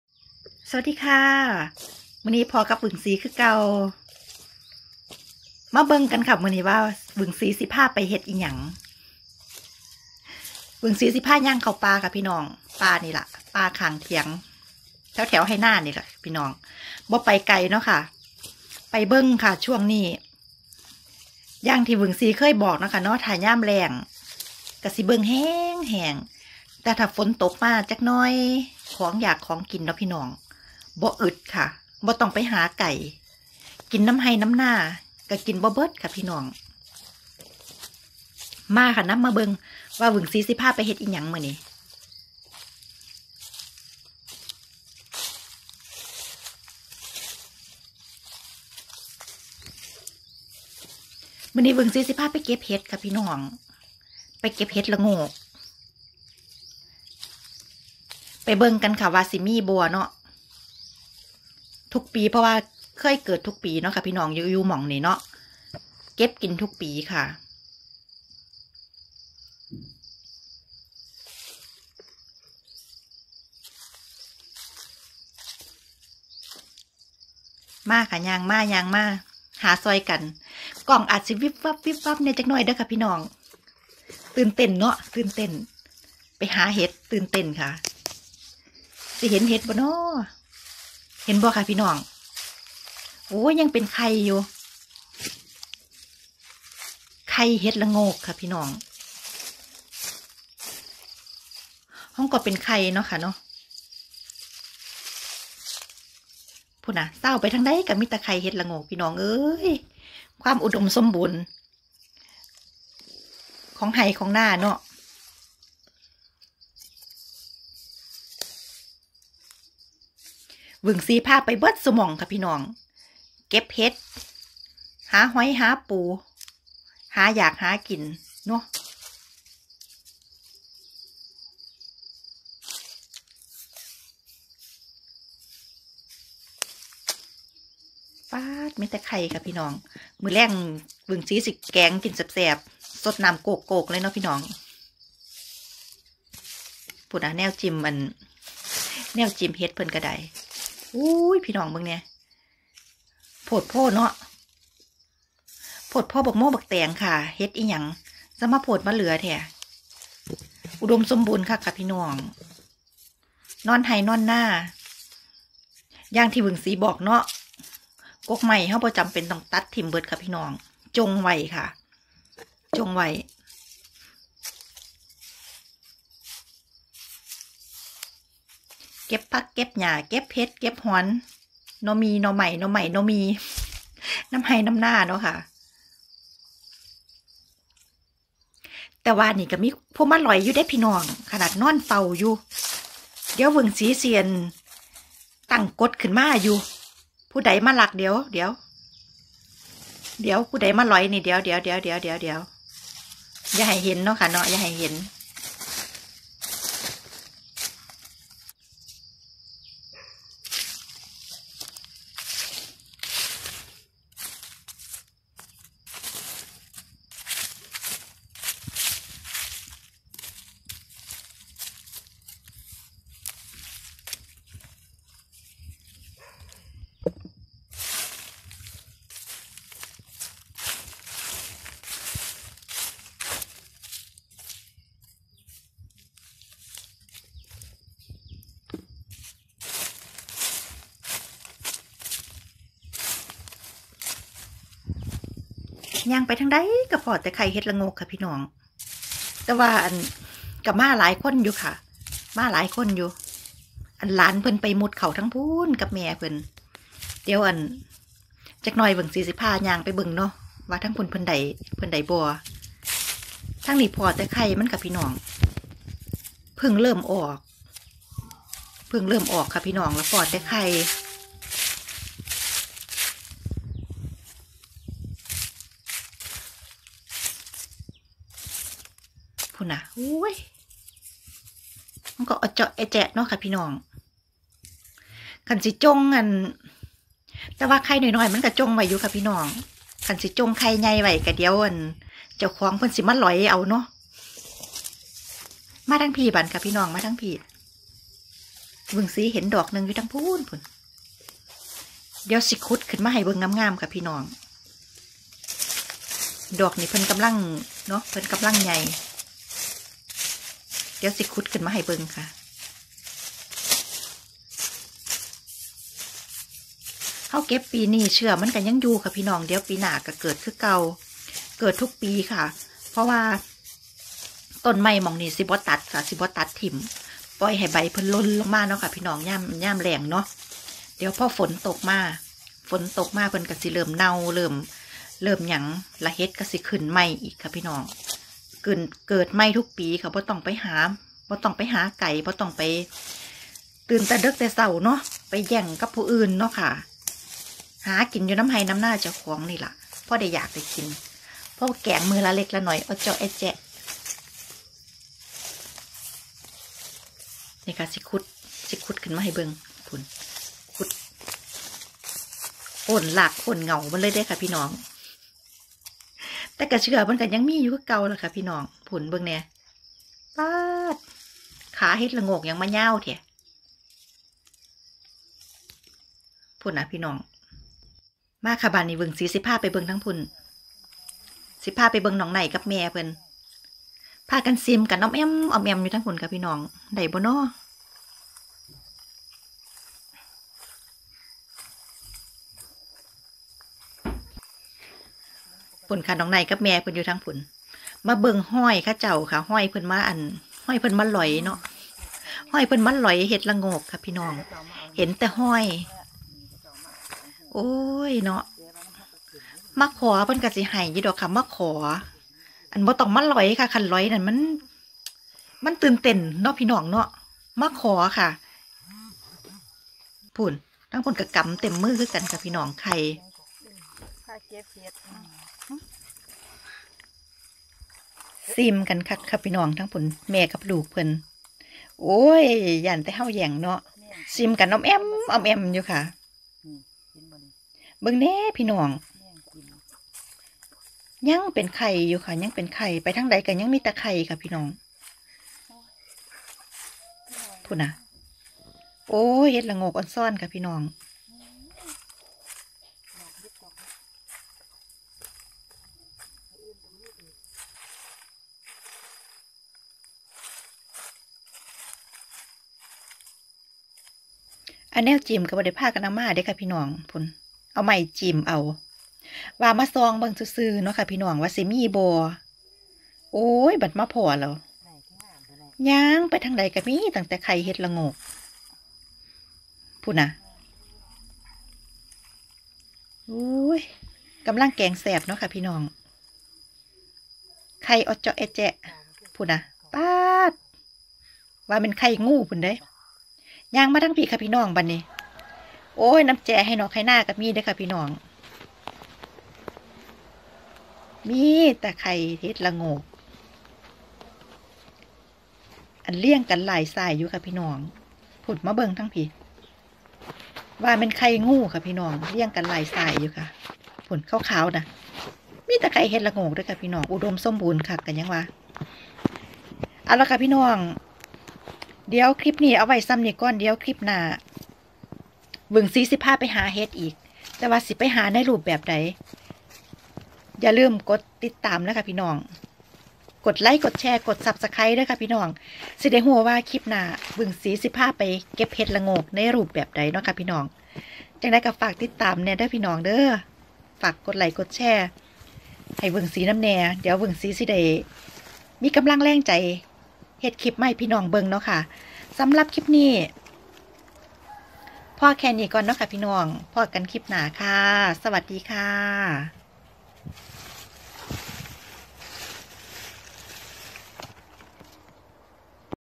เก็บเห็ดระโงก🍄🍄🍄มีแต่ไข่กำลังงาม
เก็บเห็ดระโงกป่าข้างเถียงนา